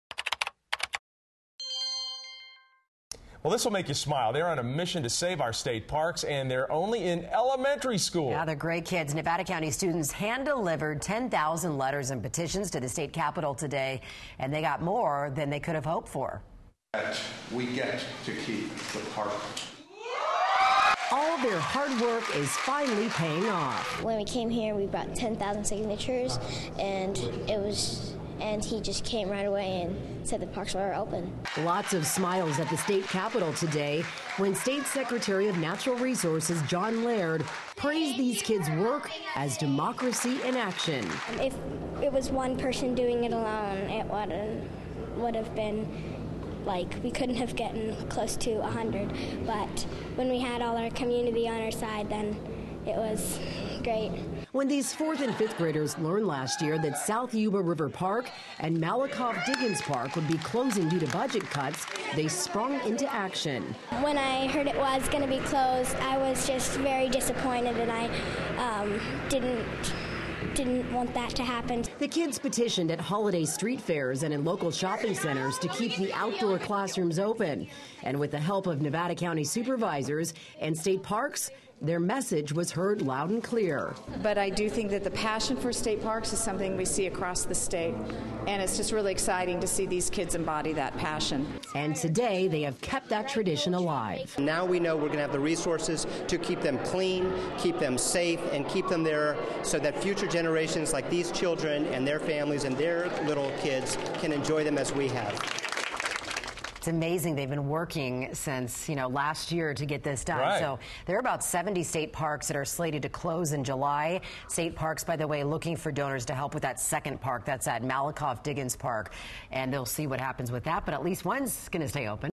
School Kids On A Mission To Save State Parks
Nevada County school kids delivered 10,000 letters and petitions to the State Capitol in an effort to save state parks.